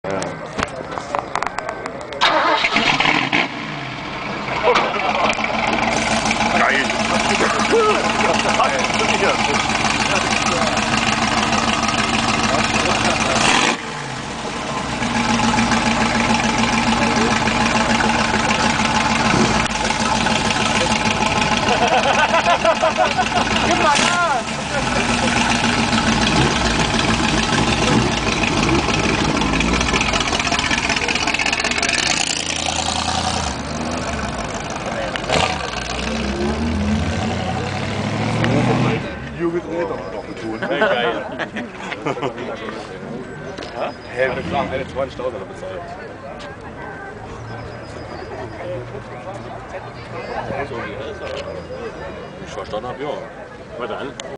Altautotreff Saar-Lor-Lux 20.03.2011 - Soundcheck Ford Modell A Hot Rod
Hier eine kleine Momentaufnahme vom heutigen (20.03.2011) Altautotreff in Merzig. Treffpunkt jeden 3. Sonntag im Monat um 15 Uhr vor dem Burger King in Merzig ...